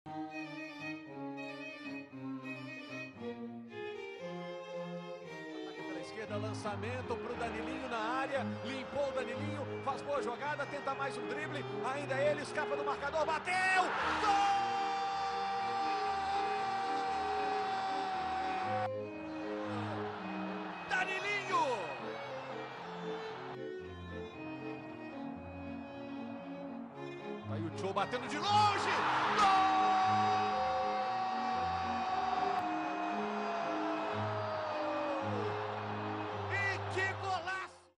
2006 GALO 2x0 Paulista
12/09/2006 - Atlético MG 2x0 Paulista SP

Estádio: Mineirão.
Público: 32.210 pagantes – Renda: R$ 188.757,50.
Competição: Campeonato Brasileiro – Série B.
Árbitro: Elmo Alves Resende Cunha, GO.
Expulsão: Marcus Vinicius (P-54').

ATLÉTICO: Diego Alves; Tony, Marcos, Lima e André Santos; Rafael Miranda (Henrique), Márcio Araújo e Marcinho; Danilinho, Marinho (Éder Luis) e Rôni (Tchô) – Tec: Levir Culpi.

PAULISTA: Victor; Marco Aurélio, Dema, Marcus Vinicius e Fábio Vidal; Rever, Gláucio (Rivaldo), Marcelo Oliveira e Glaydson; Victor Santana (Diogo) e Jaílson (Fernando Diniz) – Tec: Vagner Mancini.

GOLS: Danilinho (25’) e Tchô (80’).